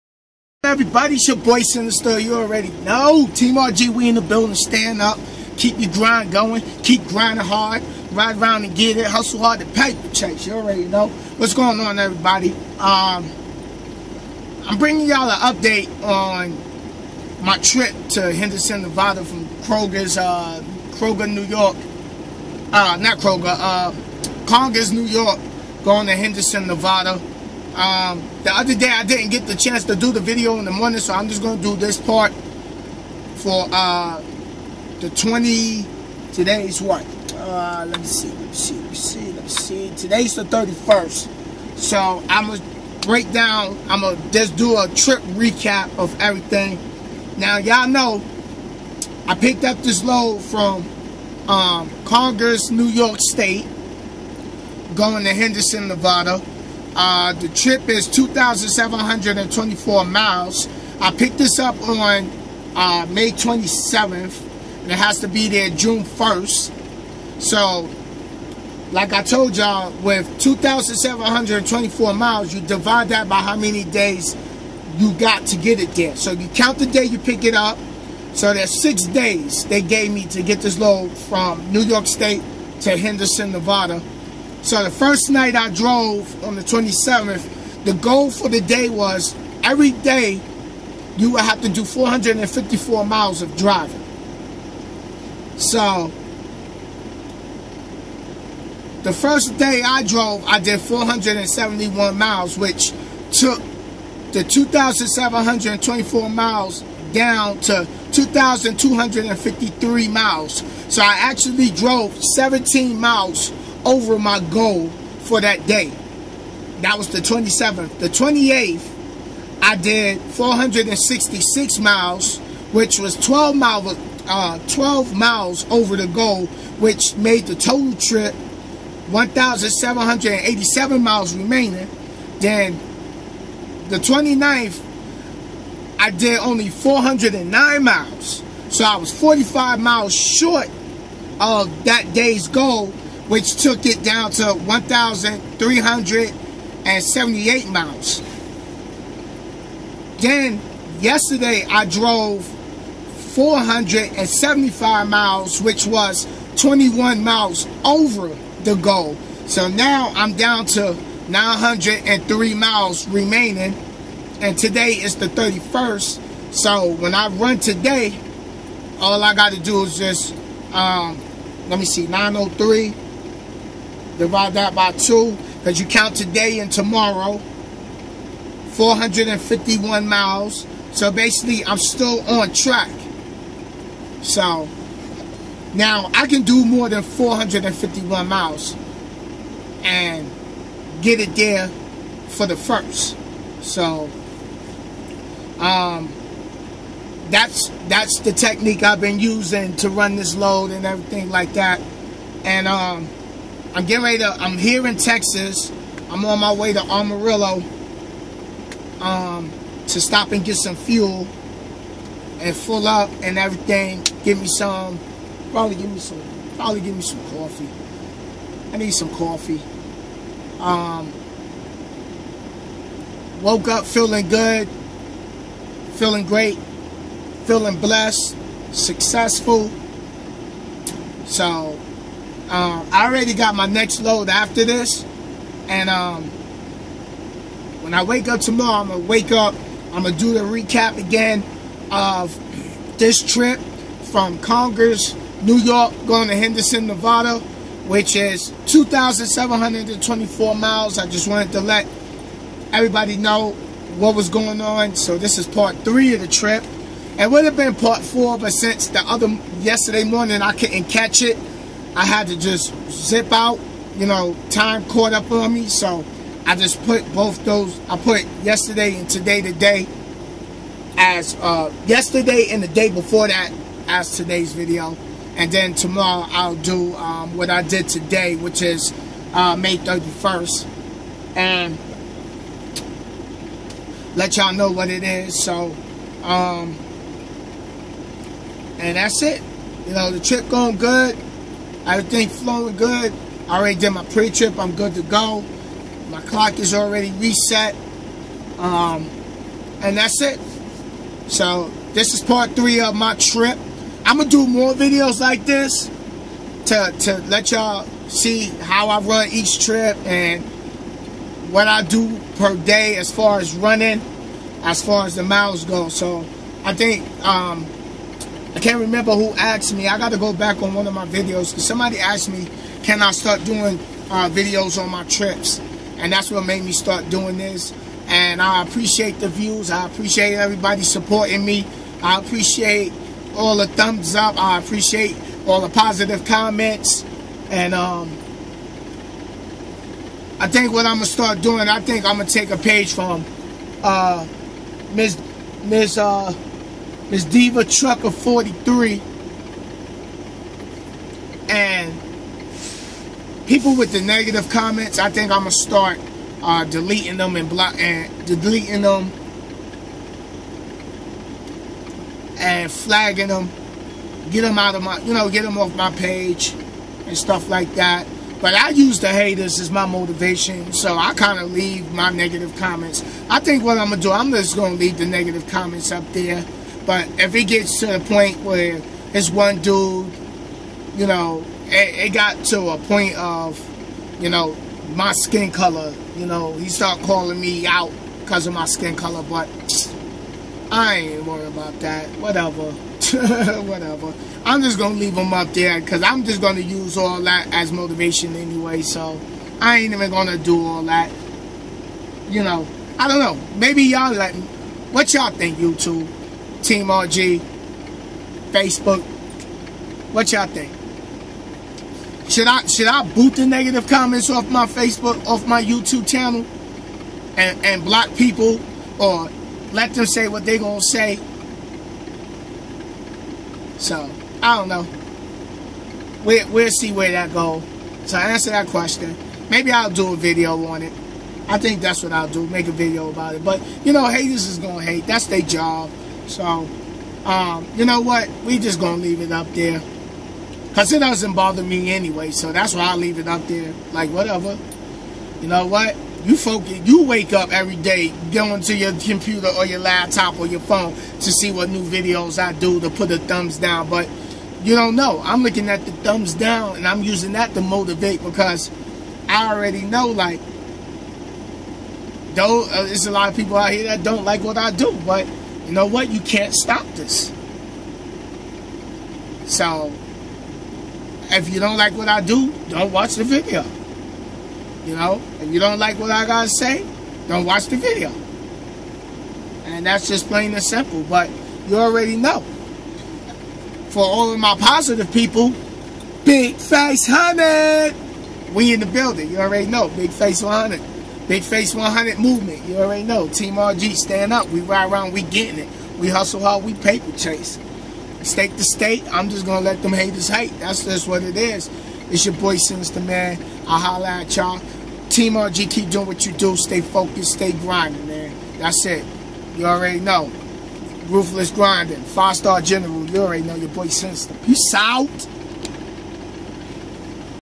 Mr.Sinnizter DaTrucker: 2,724 Mile Trip From Congers,NY To Nevada Part.3
In This Video I Break Down My Trip That's Going From Congers,New York To Henderson,Nevada Total Miles For This Trip is 2,724.
See How I Run To See How I Run This Load To Get Delivered
On Time. Please Comment,share, and like the video thank you.

TEAMRGCLOTHING.SPREADSHIRT.COM